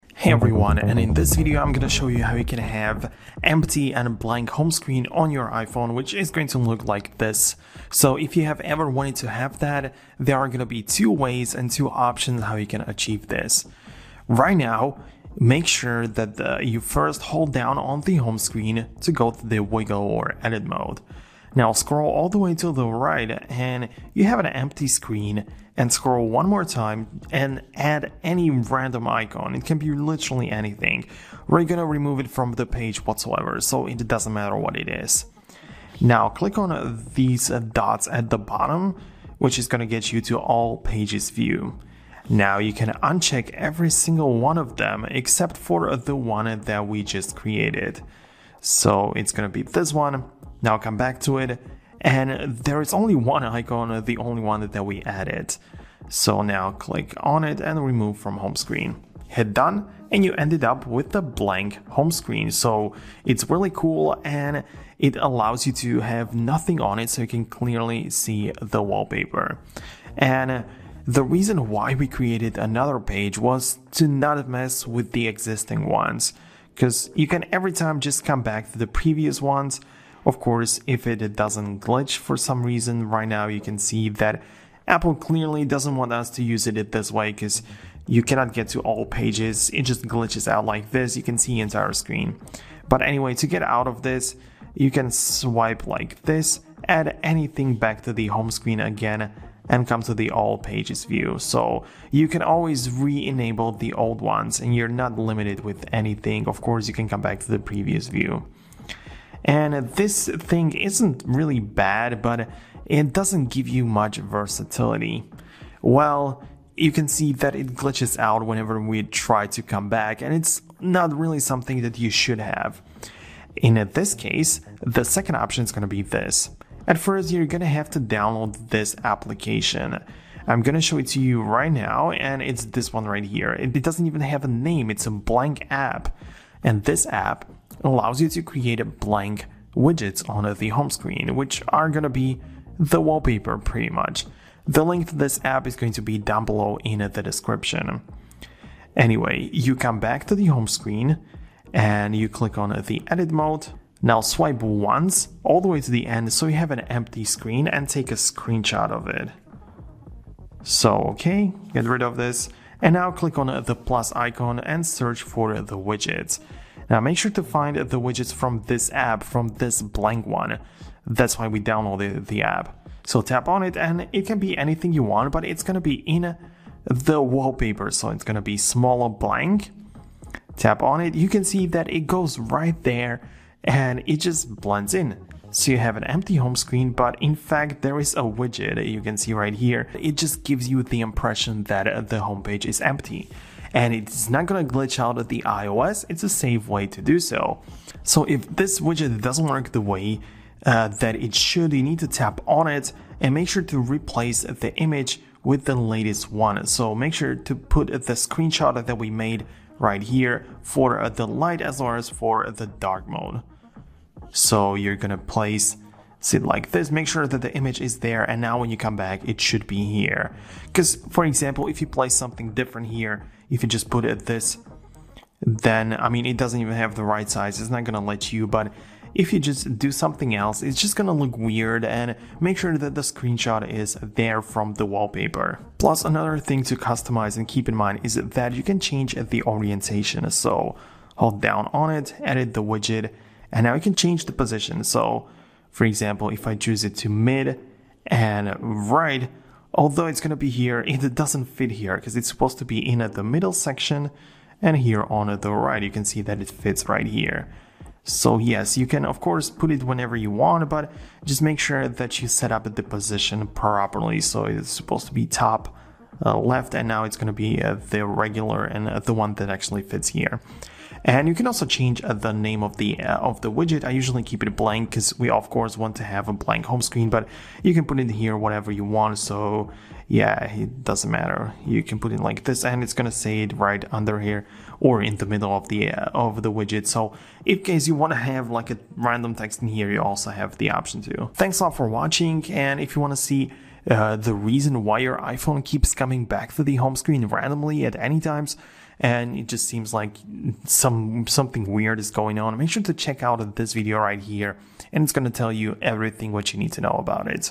How to Get a Blank Home Screen on iPhone iOS 16
For the ultimate clean view of your iPhone home screen, you'll need to have a blank or empty page for you to see nothing but the wallpaper. That's how you achieve the ultimate setup and make sure that no icons stand in the way.

Timestamps:
0:00 How do you add a blank home screen to iPhone
0:16 First Method
2:04 Second Method
4:51 Conclusion